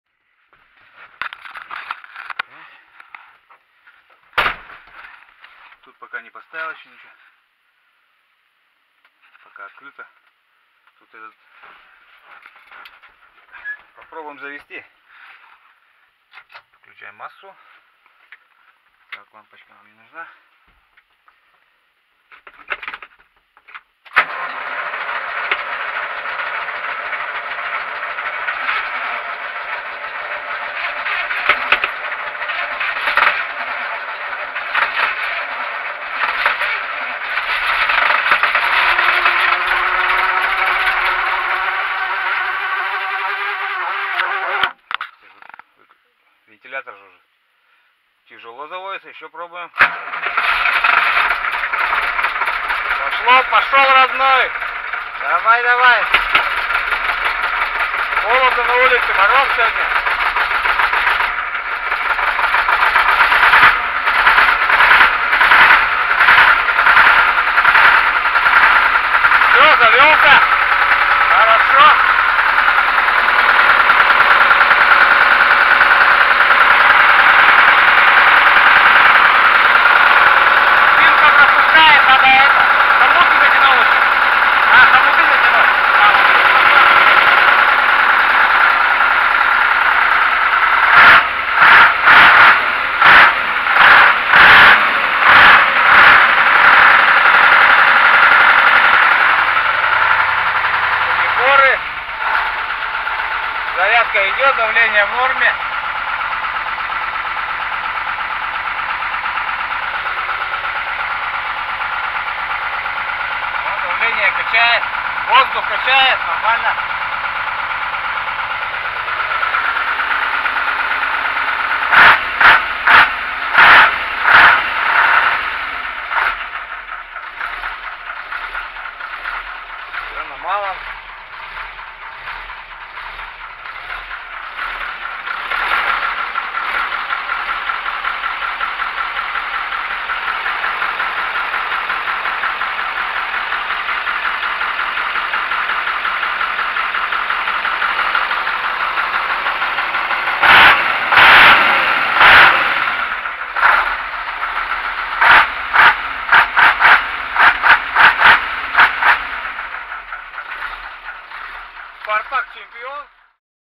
Завожу Урал ЯМЗ-238, после ремонта, зимой.
Запускаю двигатель на Урале лесовозе зимой, после замены заднего сальника коленвала.
Пермский край Красновишерск Губдор 26 ноября 2016